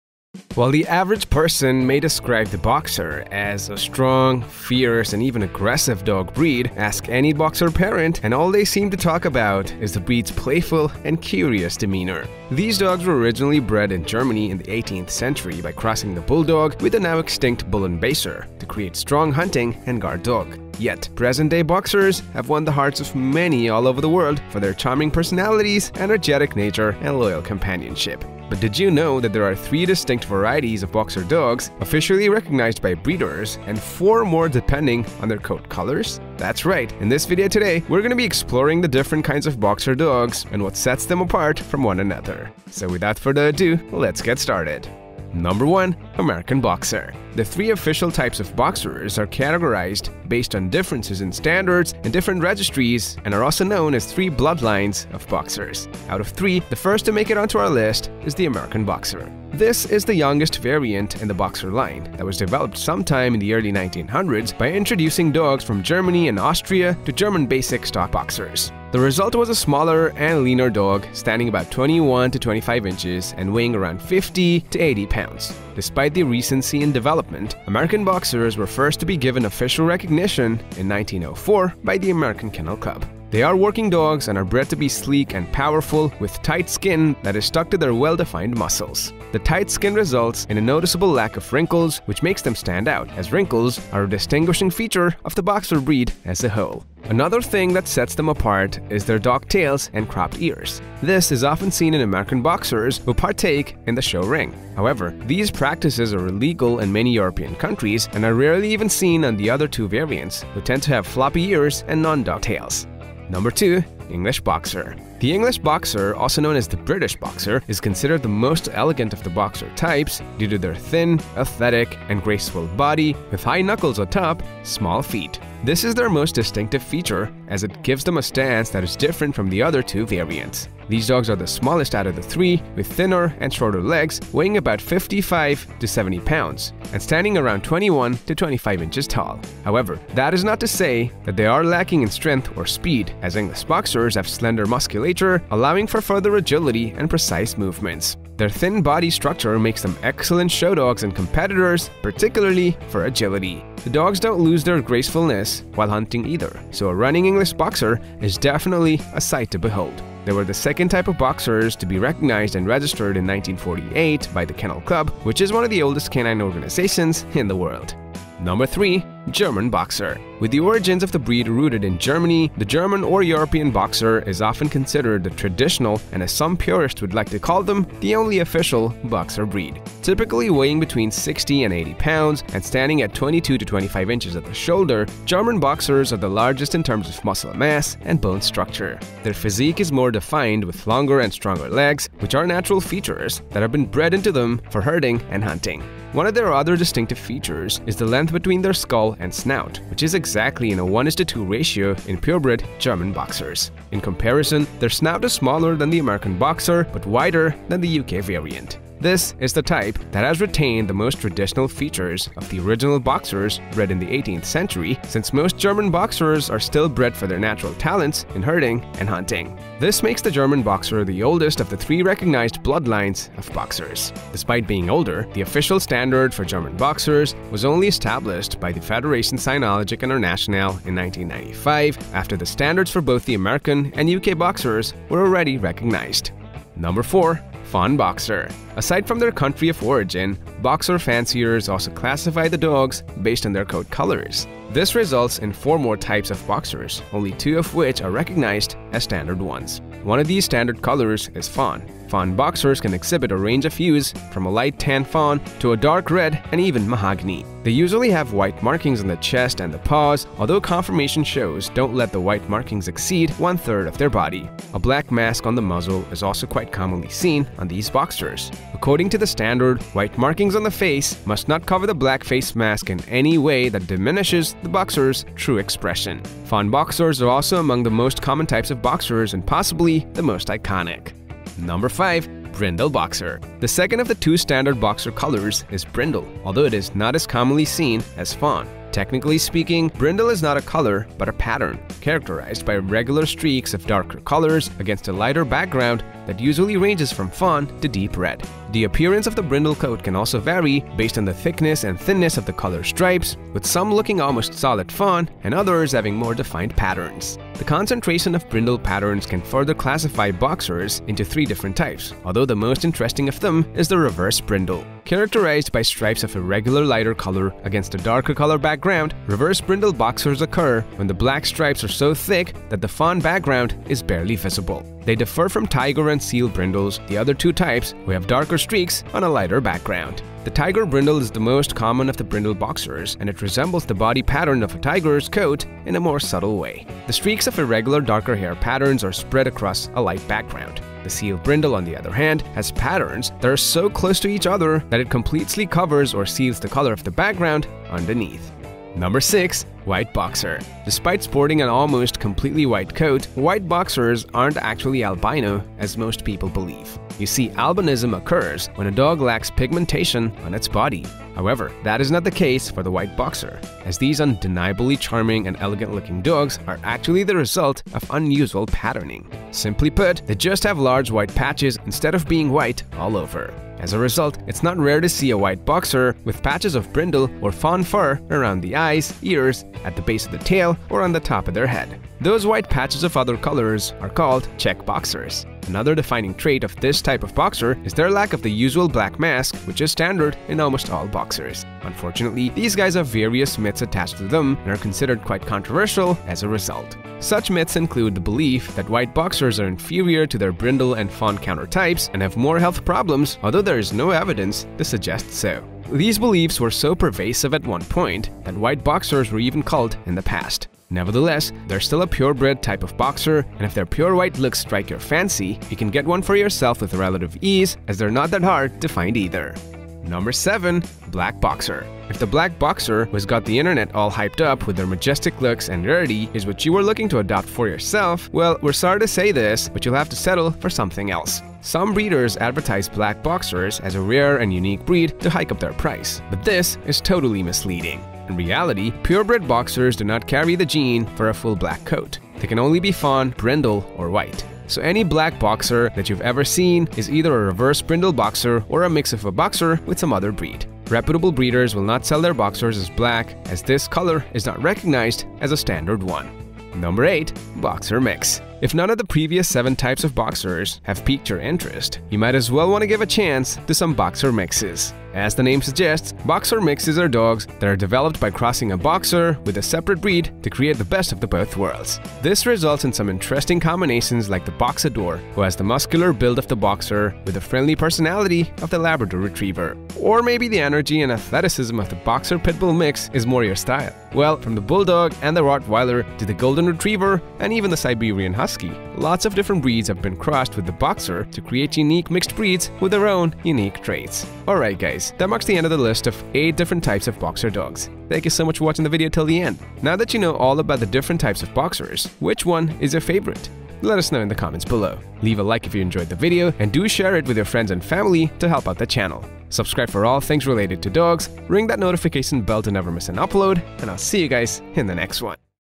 BOXER TYPES - 8 TYPES OF BOXERS
👉 Chapters:
0:00 Intro
0:45 American Boxer
1:55 English Boxer
2:50 German Boxer
3:57 Fawn Boxer
4:43 Brindle Boxer
5:50 White Boxer
7:01 Black Boxer
7:37 Boxer Mix
8:15 Outro